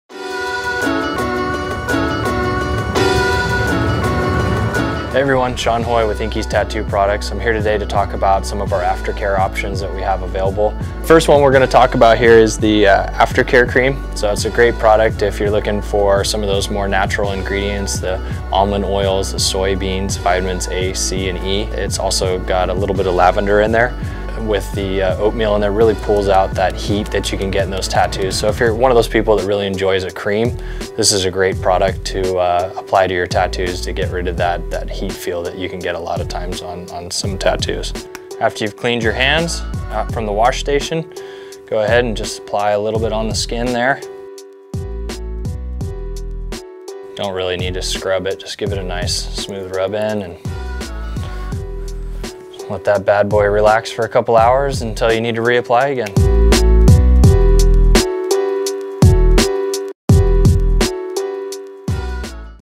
Sean Hoy - Aftercare Cream
Sean Hoy talks about the benefits of the Ink-Eeze aftercare cream!